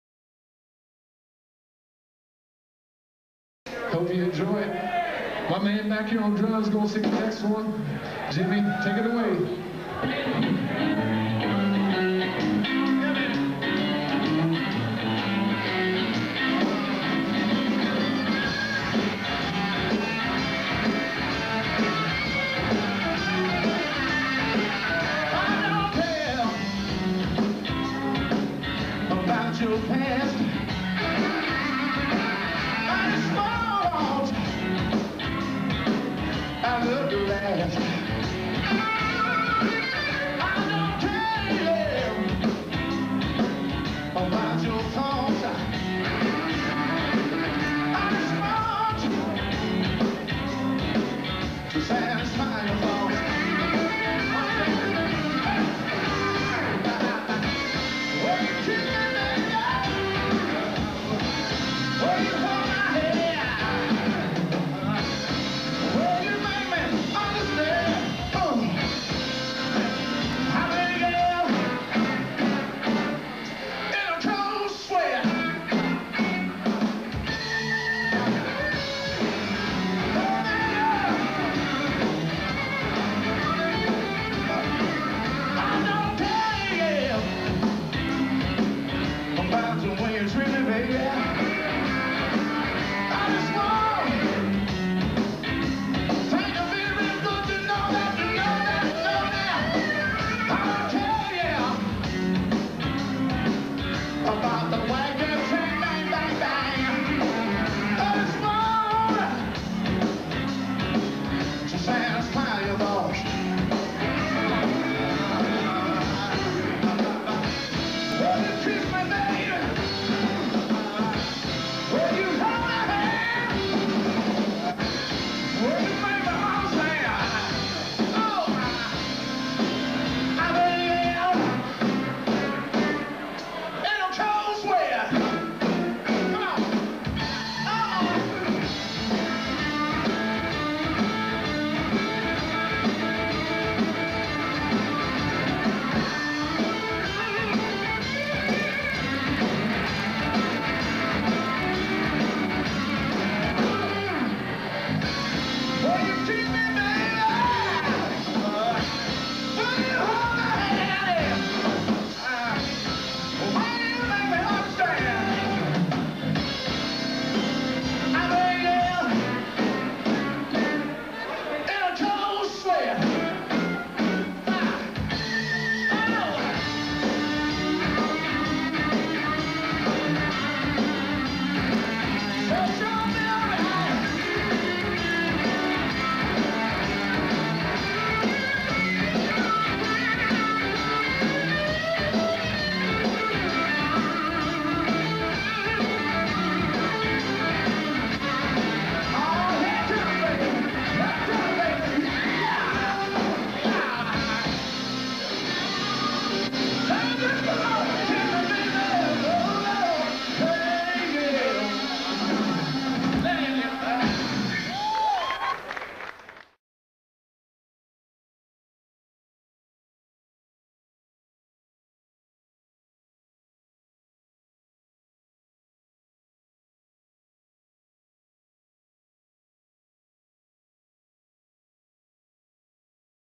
RHYTHM WHEELS March 18th 1998
Live at Dreadlocks in Lexington, Kentucky...The Rhythm Wheels perform an old James Brown standard. Chris Jones on lead guitar, Rod Zimmerman on rhythm guitar, Ron Perry on bass and Jimmy Ellison on drums and vocals.